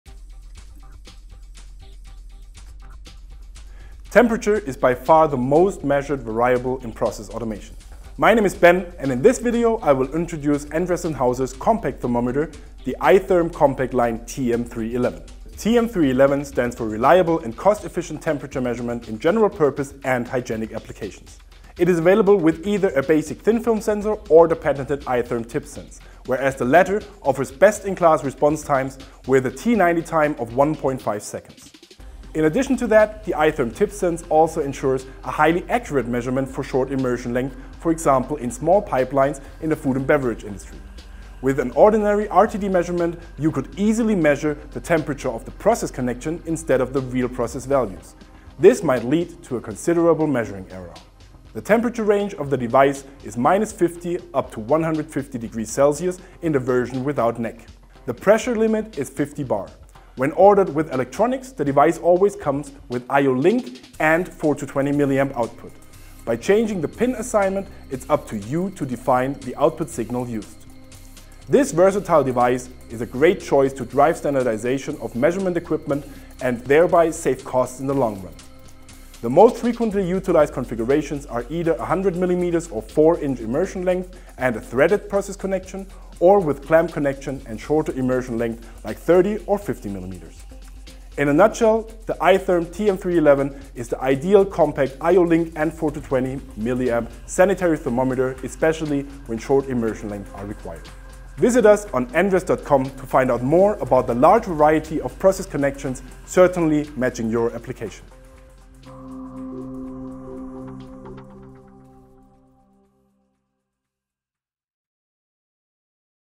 iTHERM CompactLine TM311 - Explained in under 2 minutes
In this video we give you a quick overview about most important features of our compact, accurate and fast RTD thermometer iTHERM CompactLine TM311.